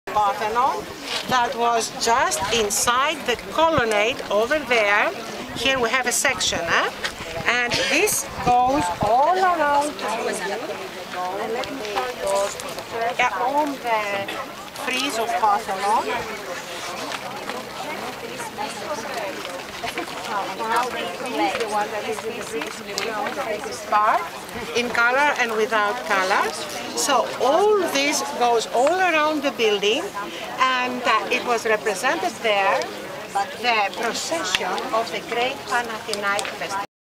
Athens, Greece tour of Acropolis and Parthenon
Tour guide talking about ruins of Acropolis and Parthenon in Athens Greece.